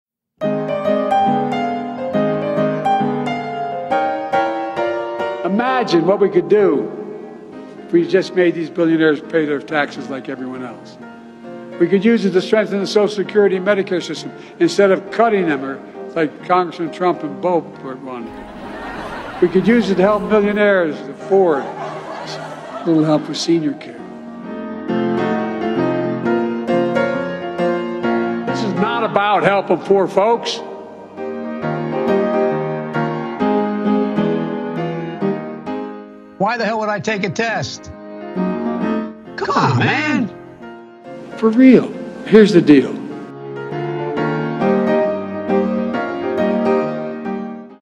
81 million Votes
Joe Biden gives another terrible speech in which he refers to "Congressman Trump," whoever that is.  This debacle took place at CS Wind in Pueblo, Colorado.  Let's go Brandon; c'mon man!